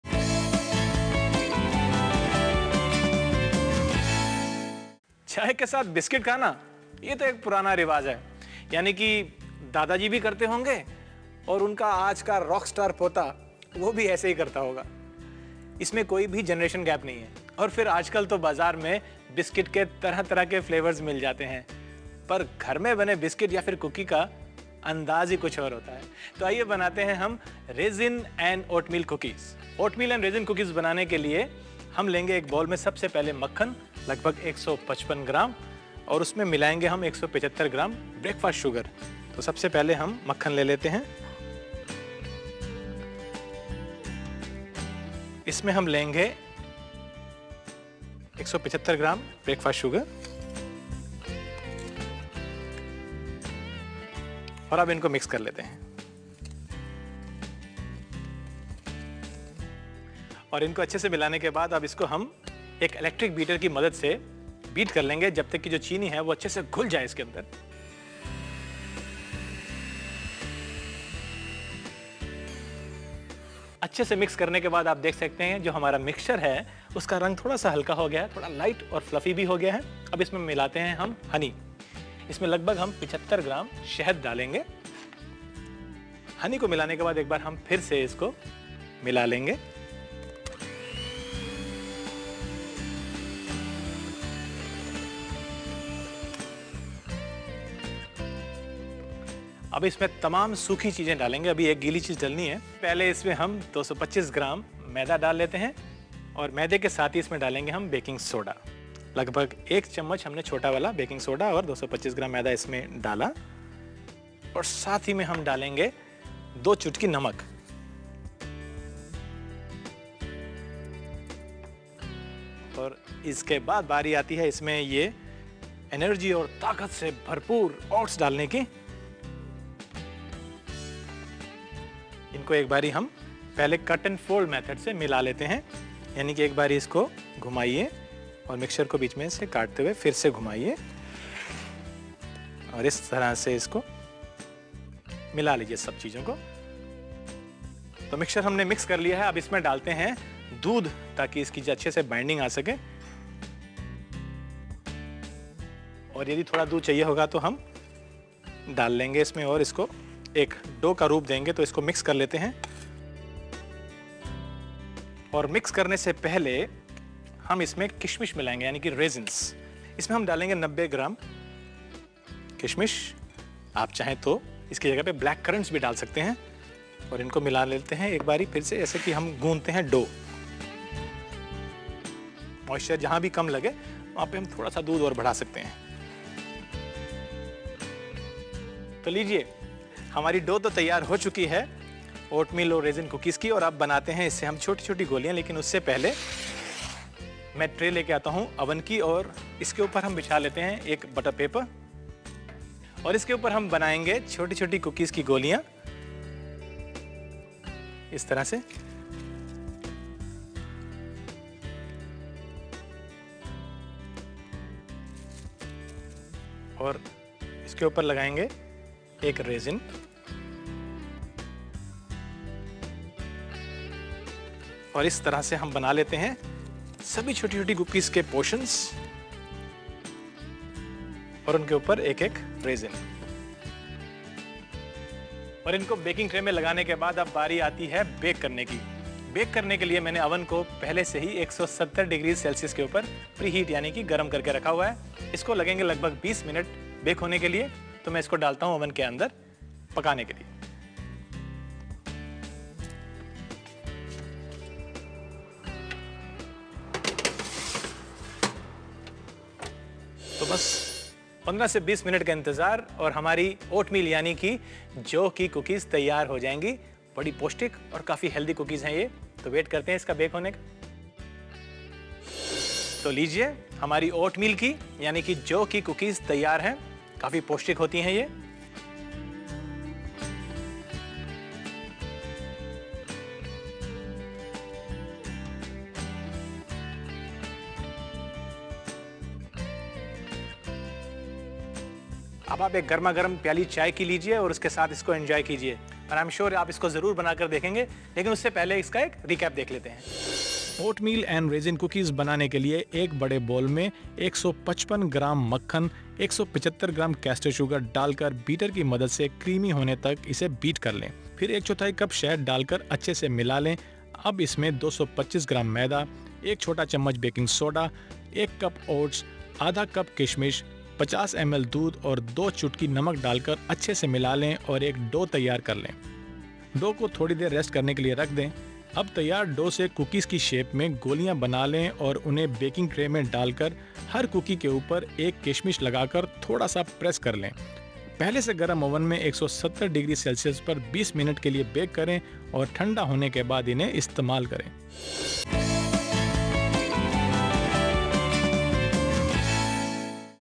Oatmeal & Raisin Cookies - Tea Time
Chef Rakesh Sethi teaches the viewers how to make innovative and easy dishes that are perfect with during tea time. From Indian to fusion, these recipes are easy to make and a delight to have on the menu!